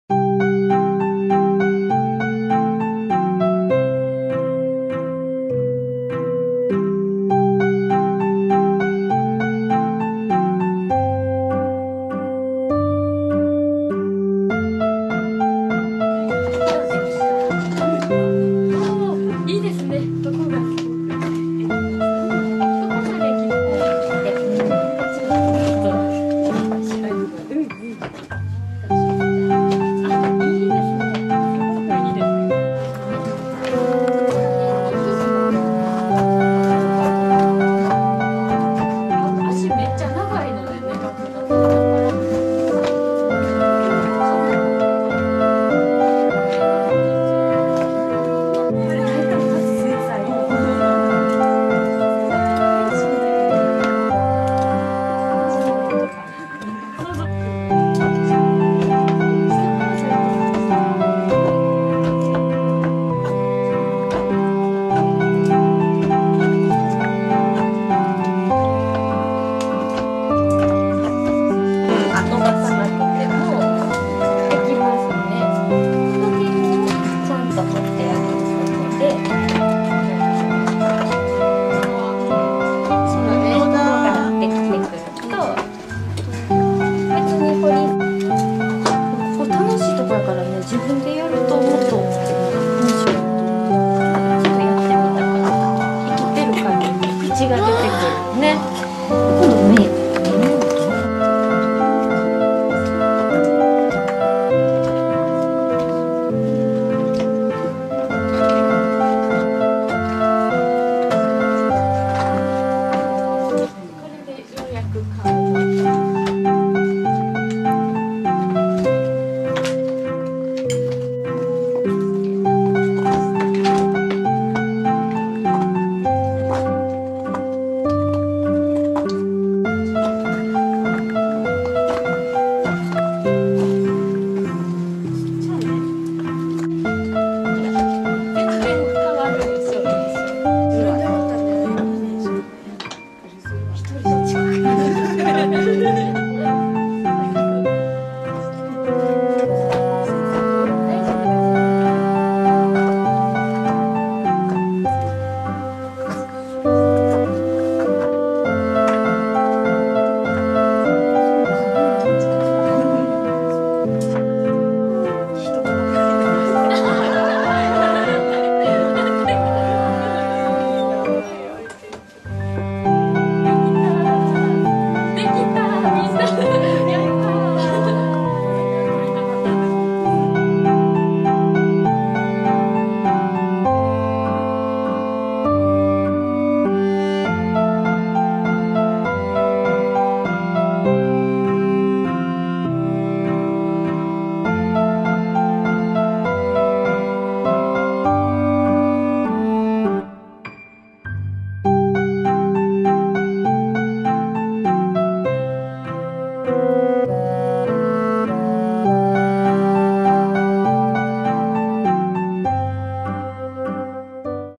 Let's try to make a tiny sculpture of Dromedary!
ちいさなラクダを彫ってみよう！
はしもとみおの木彫りワークショップ

ちいさなクスノキを使って、動物の彫刻に挑戦、
今回は、ヒトコブラクダを３時間でつくりました。